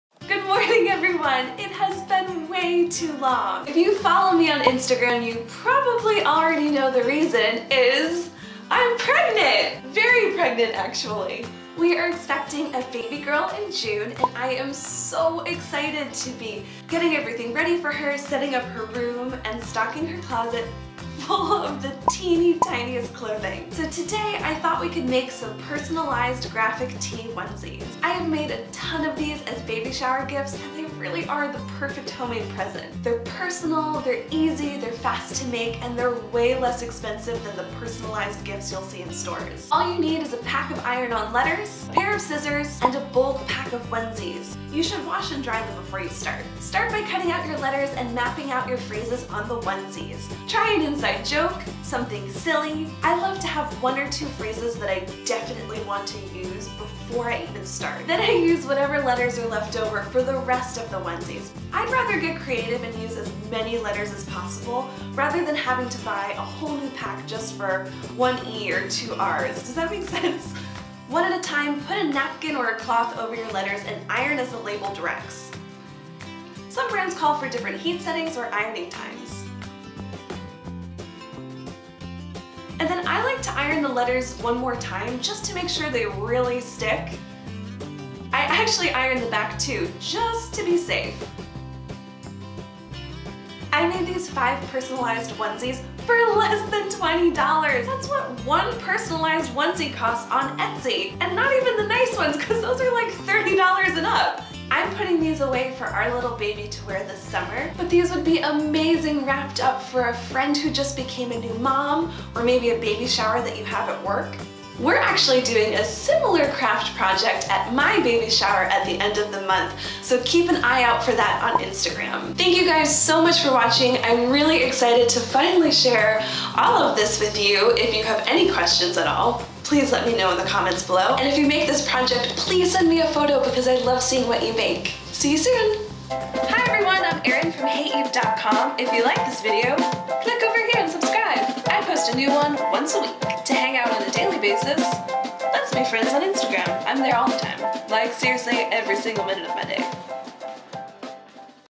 DIY Personalized Onesies | Homemade Baby Shower Gift
Looking for the perfect DIY Baby Shower Gift!? These personalized onesies are an easy homemade gift for any new mom. In this video, I’m crafting my own set of teeny-tiny bodysuits for our baby girl, due in June! SURPRISE!!! Don’t miss this easy tutorial for homemade baby clothes that are cheap & cool xox

If you'd like more easy recipes & DIY ideas, follow me on...